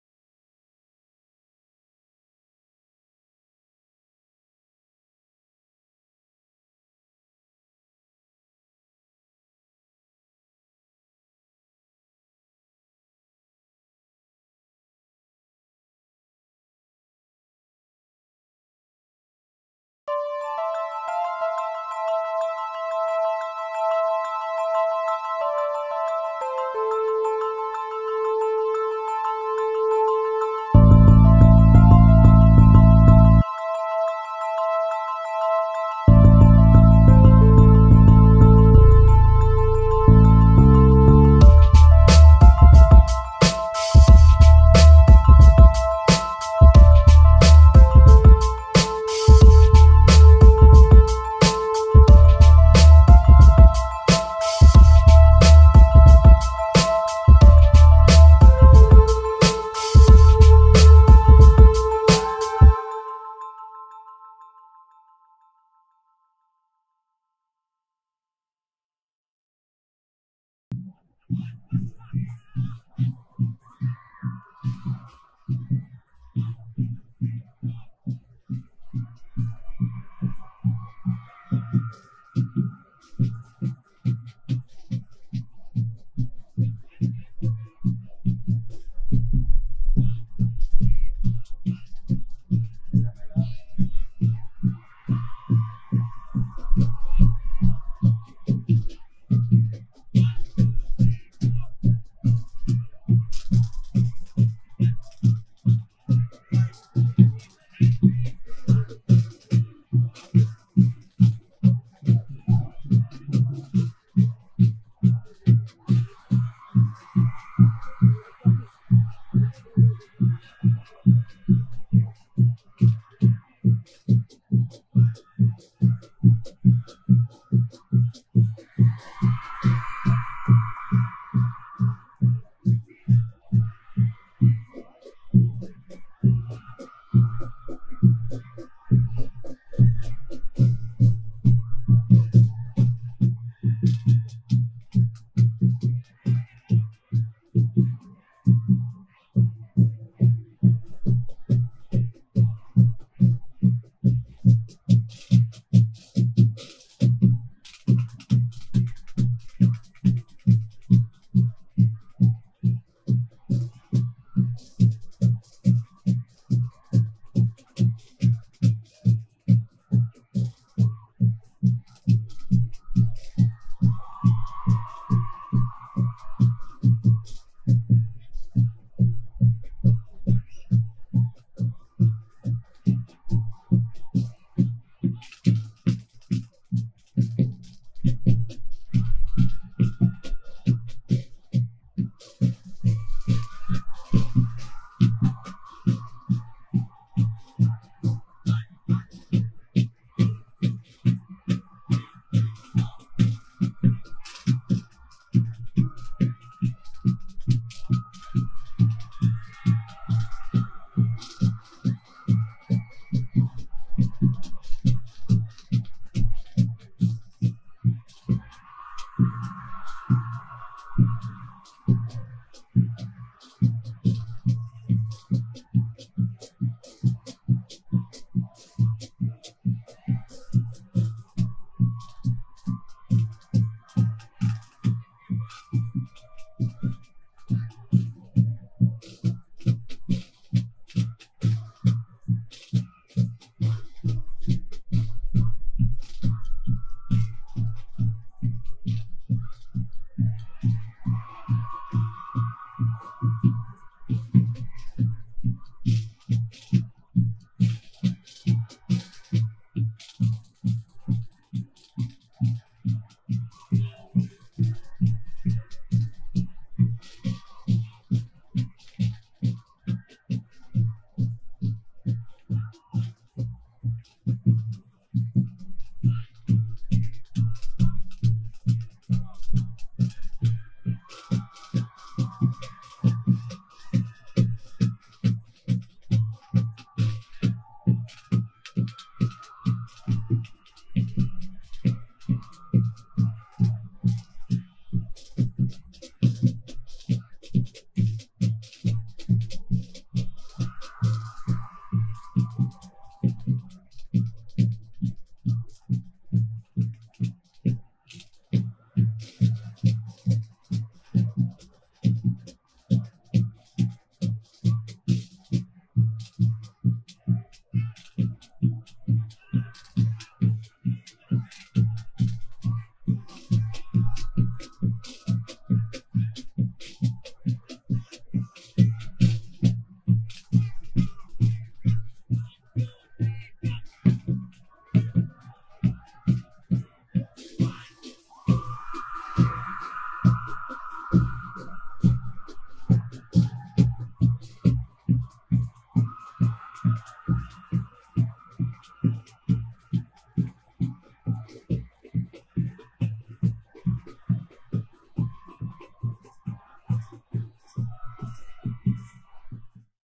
YMSC EMP 2013
Young Men Sporting Club aka" Marching Panthers" performing their 2013 Exhibition March Pass at the CMCB Grand Finale, Athlone Stadium